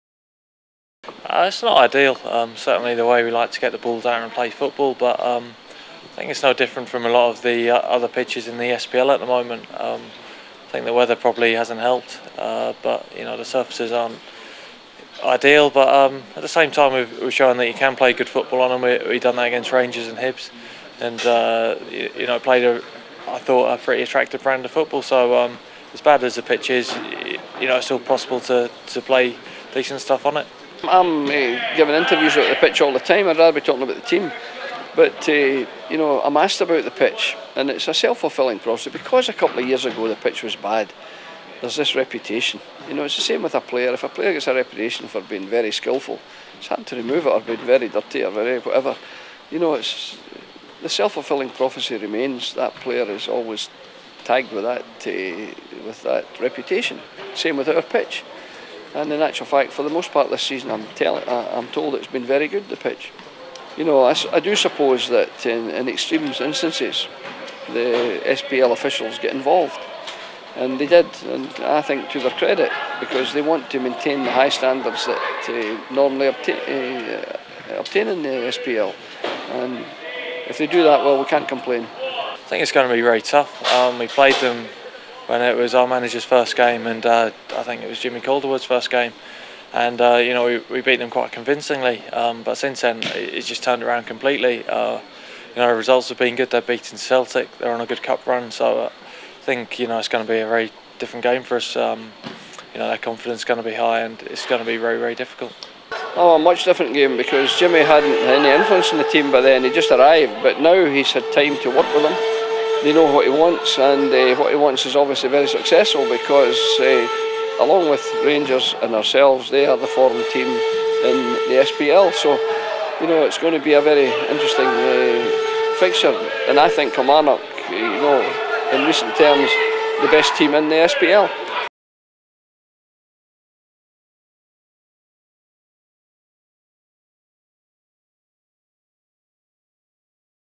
Motherwell v Kilmarnock Clydesdale Bank Premier League Preview
Motherwell striker John Sutton and manager Craig Brown take time out of training to preview this weekend's Clydesdale Bank Premier League clash with Kilmarnock at Fir Park